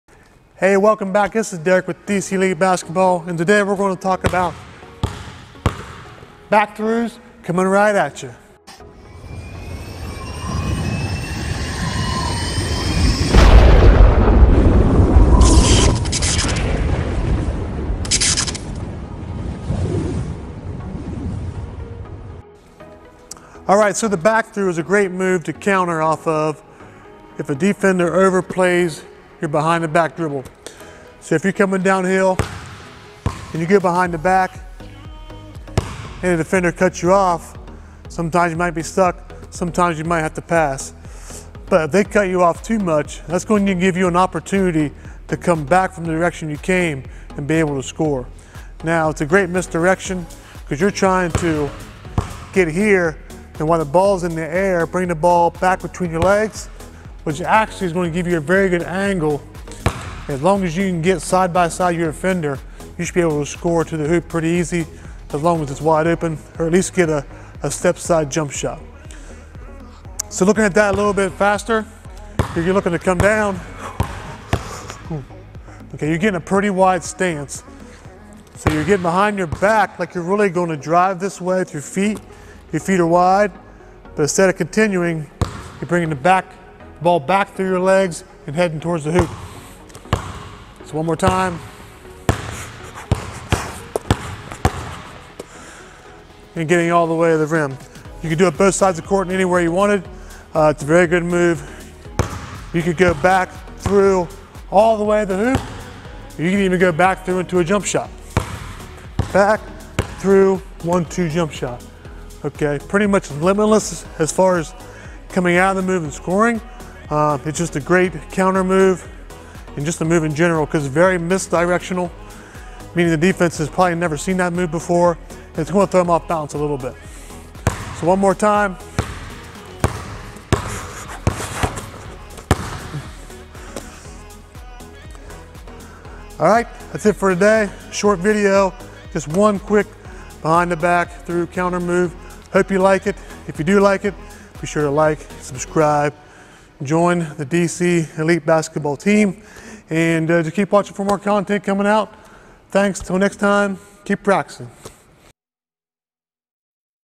BACK THRU-Behind the back counter
Back Thru is a great counter when a defender takes your angle away when trying to attack out of a behind the back dribble.

Also can be used as a set up move to get inside position on a defender as well.